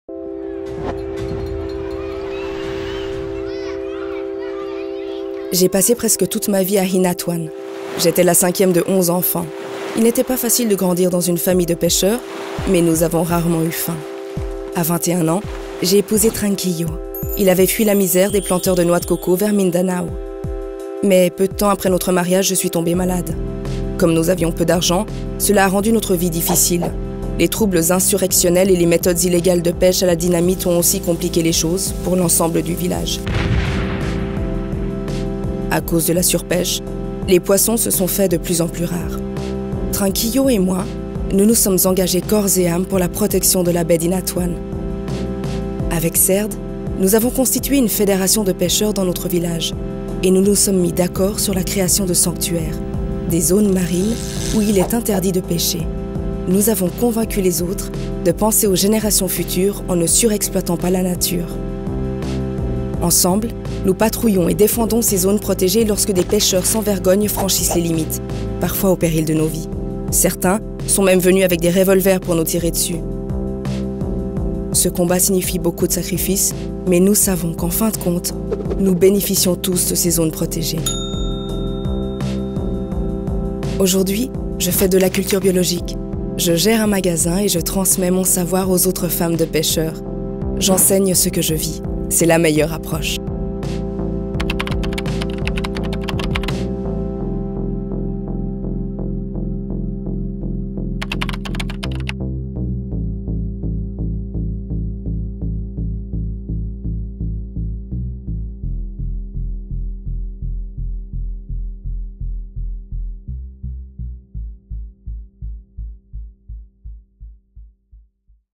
A Voice in Rio -- Fidelina Bagusan-Yana (Philippines)
et élisez le projet qui sera présenté au monde entier lors du sommet de Rio.

Fidelina Bagusan-Yana (Philippines): Vit de l'agriculture bio et gère un magasin